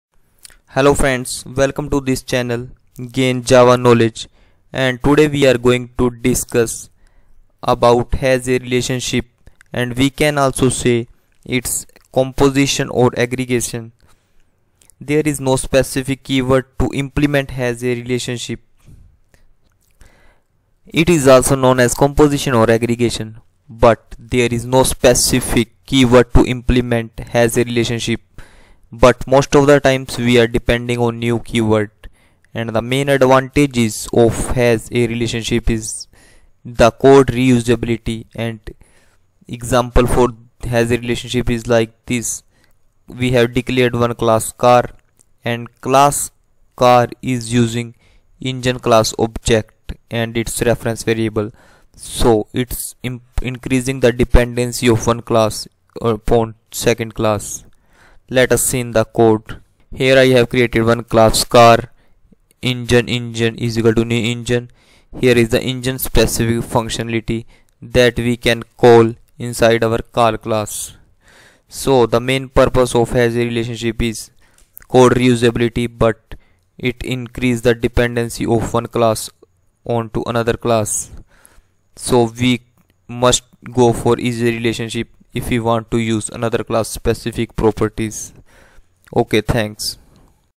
HAS-A Relationship | Composition or Aggregation in Java
Hi Friends,

Welcome to this channel Gain Java Knowledge. We are providing best content of Java in videos.

In this video we will discuss about has-a relationship in java. or composition and aggregation in java.